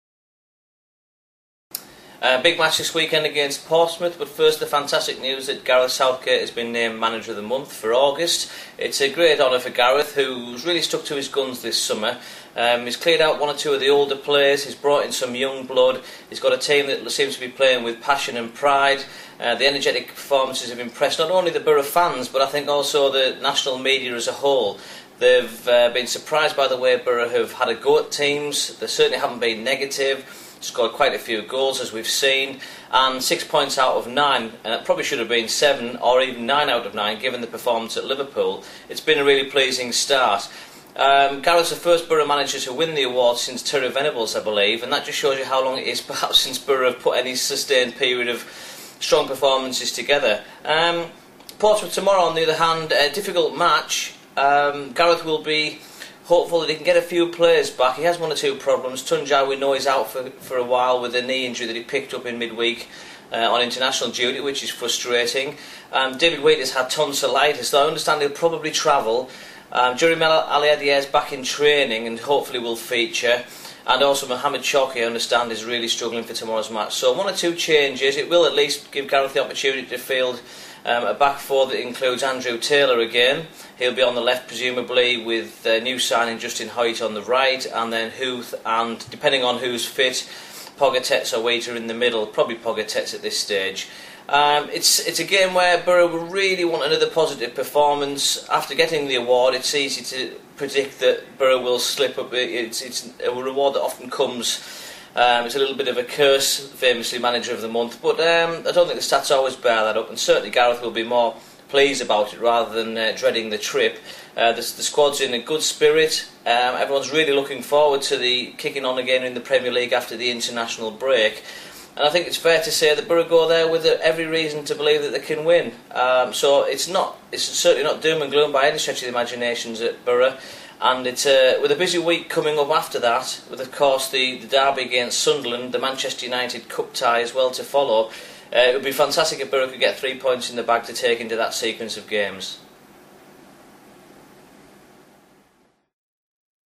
Philip Tallentire - Portsmouth preview
Philip Tallentire looks ahead to Boro's match with Portsmouth at Fratton Park.